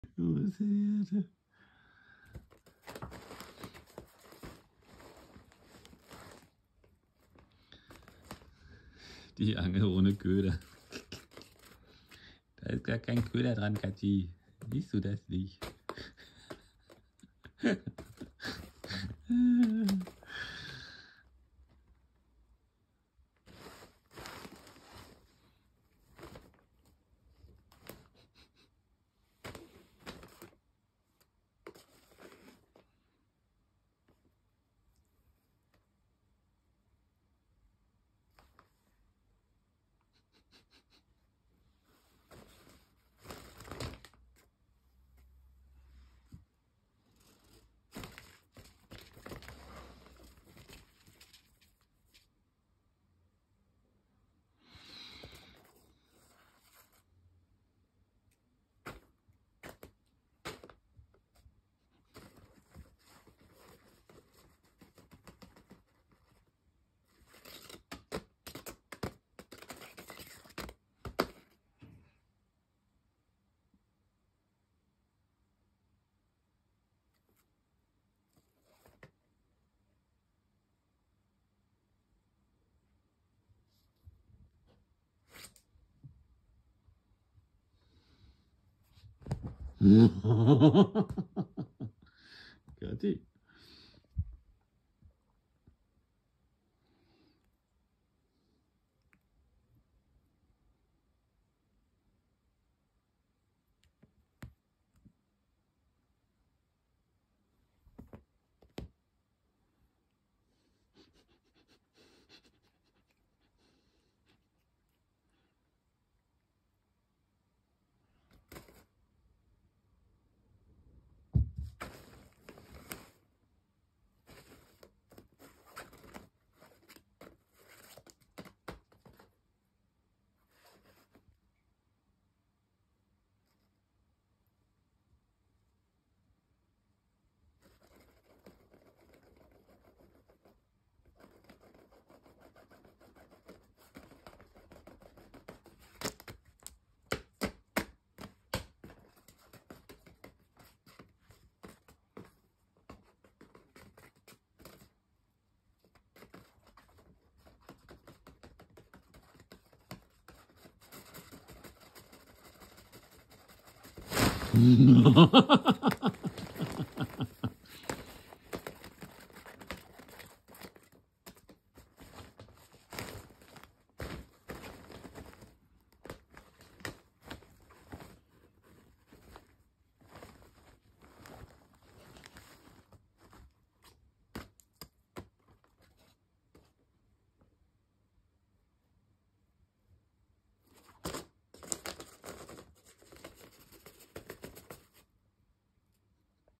Katzi spielt mit der Angel ohne Köder
Mein Katzi Lilly spielt mit ihrer Angel ohne Köder es super lustig und sehr süß viel Spaß damit. Danke fürs schauen.

Gefilmt mit meinem IPhone.